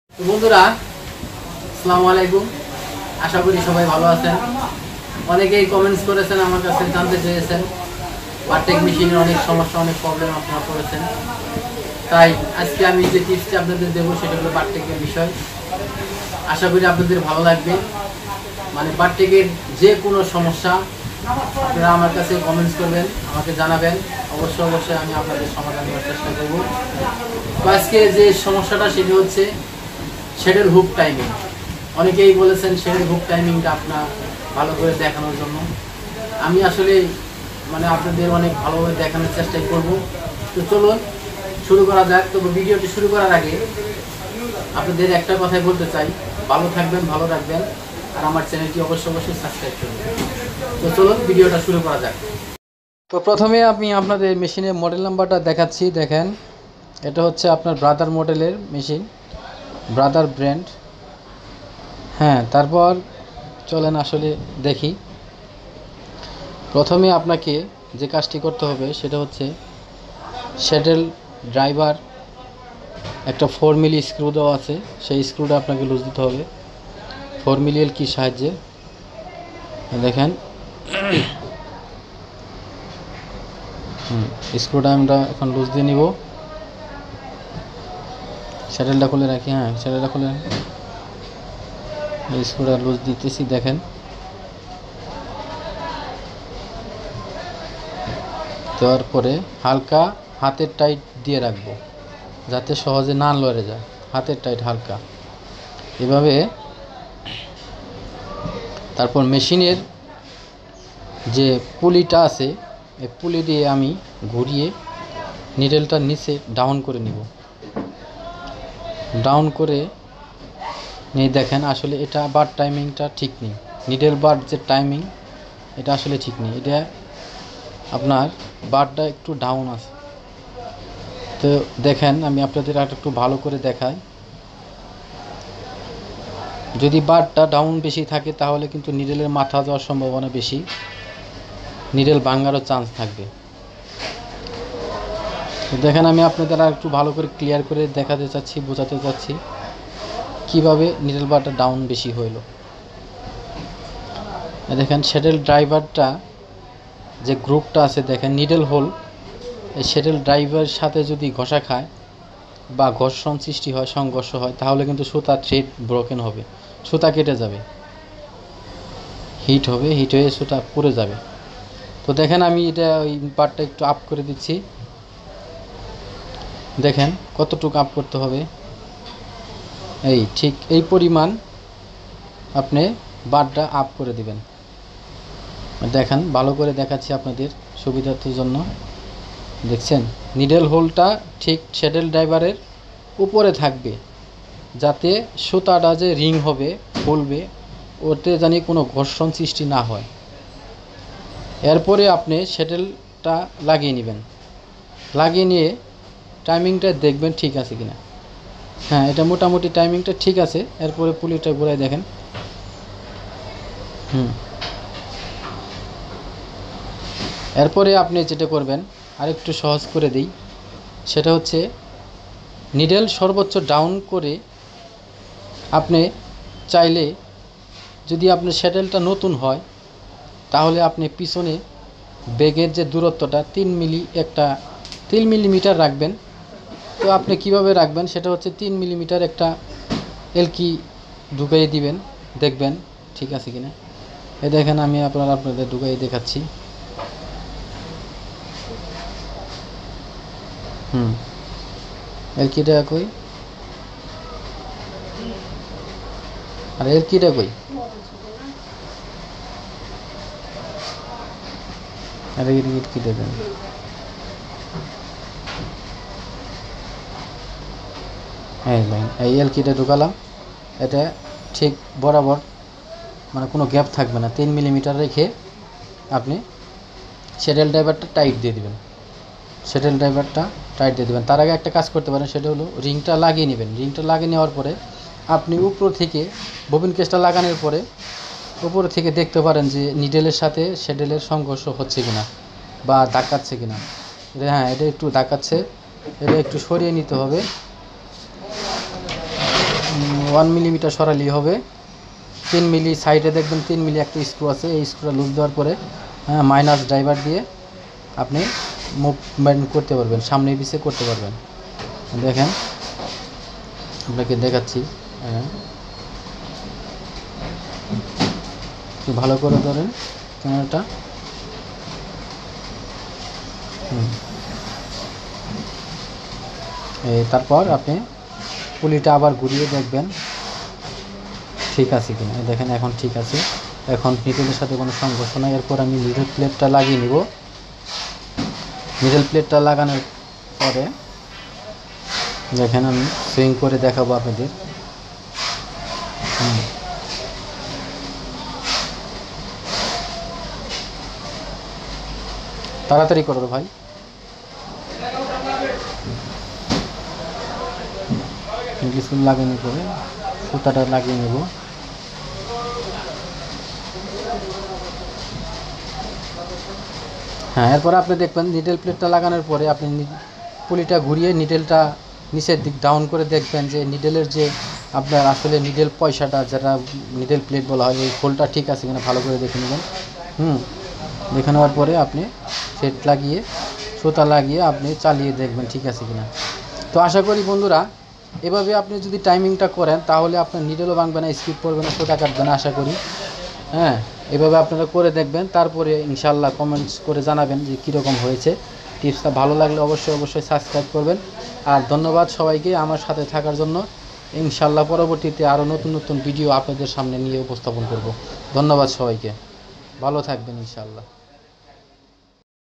BARTEK MACHINE SETTLE HOOK PERFECT TIMING MODLE KE-430FS-05
আসলে অনেকেই এই টাইমিং টা জানেনা তাই মেশিন ঠিক করতে অনেক সময় লাগেidea tv